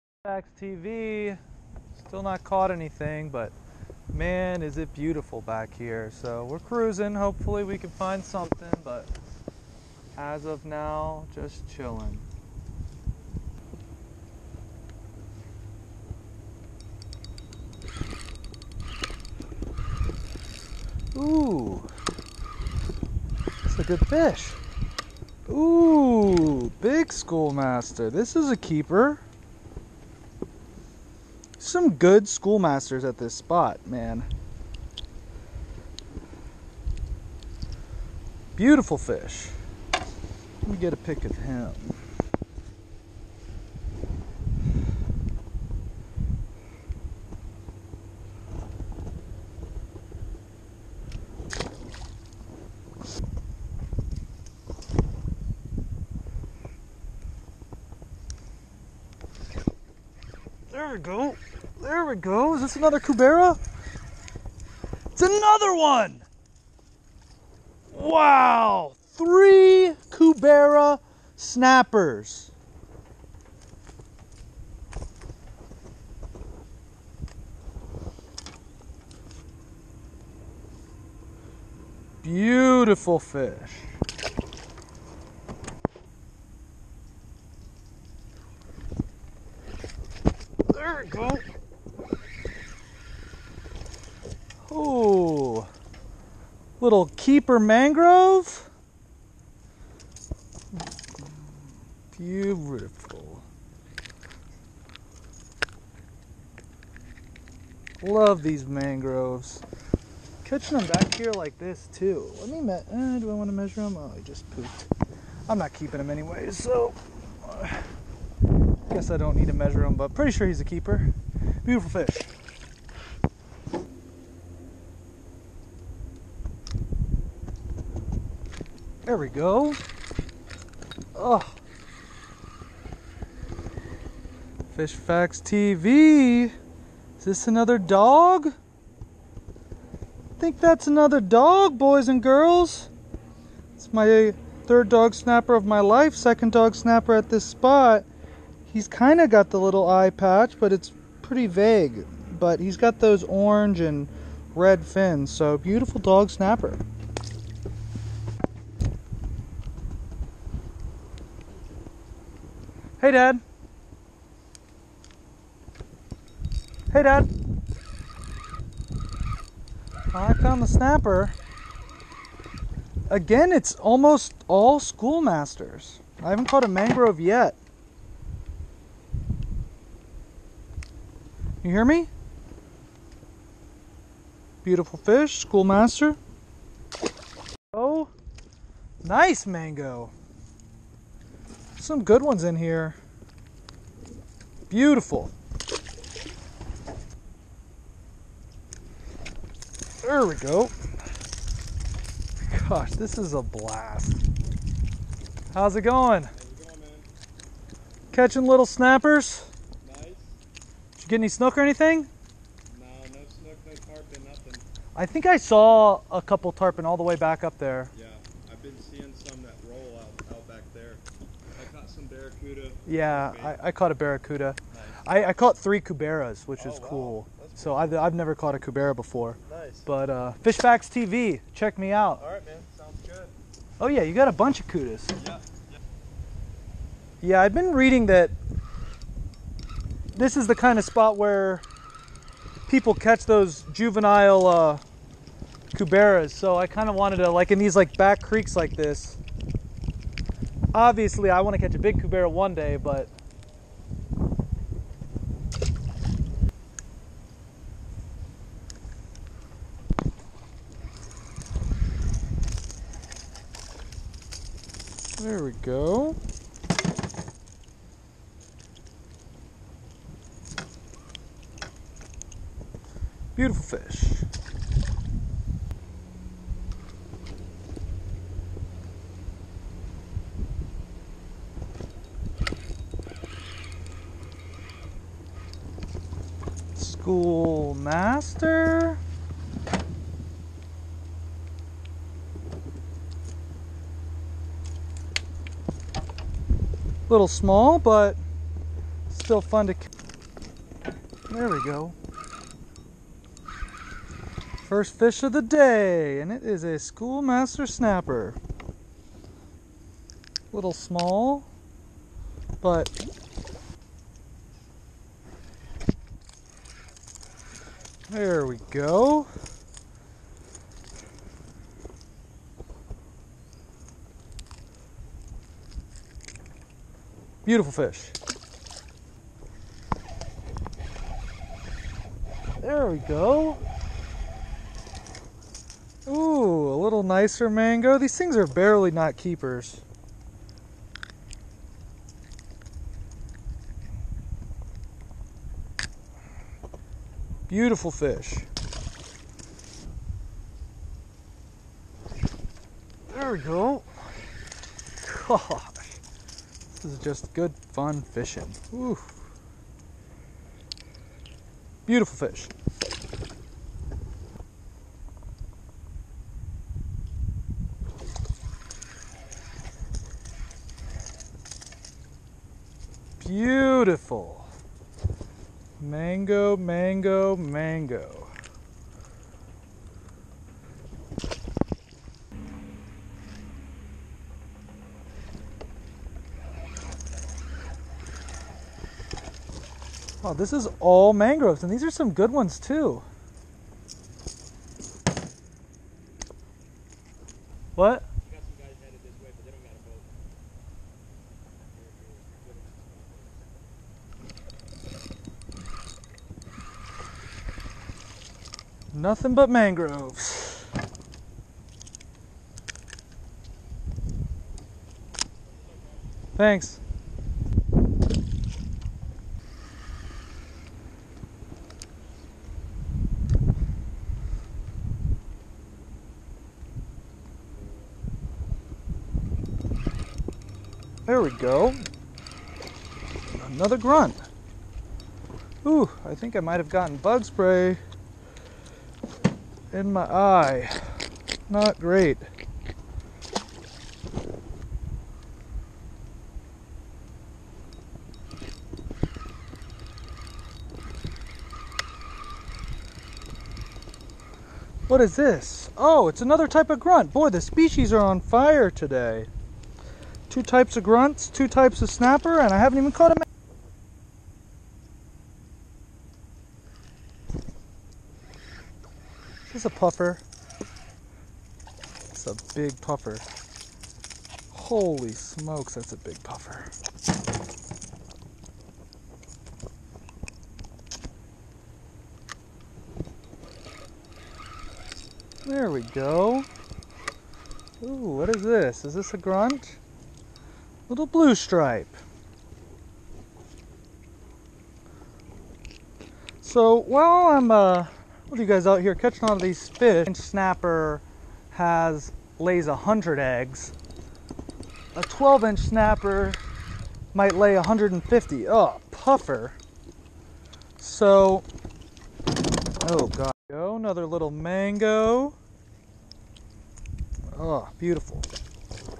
How to Mangrove Snapper Fish on Backwater Biscayne Bay Bridges (Miami)
Fishing for Mangrove Snapper on Bobbers.  Also Schoolmaster, dog, and cubera.